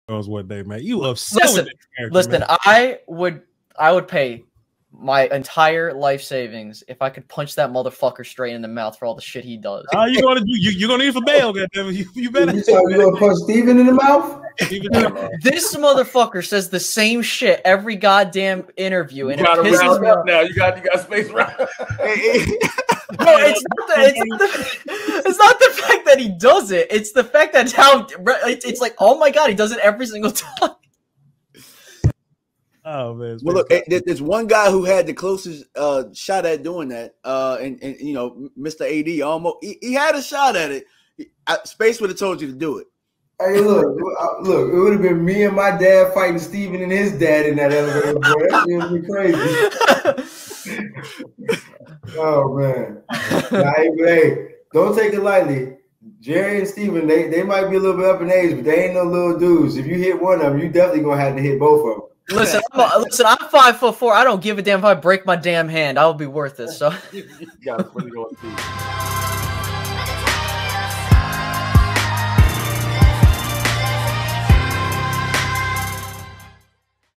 (FUNNY!) @SpaceCowboyMED Wants to Punch Stephen Jones in the Mouth 😂😂😂
vochlombardi Voch Lombardi instigates as Space Cowboy Media unveils his wish to punch Stephen Jones in the Mouth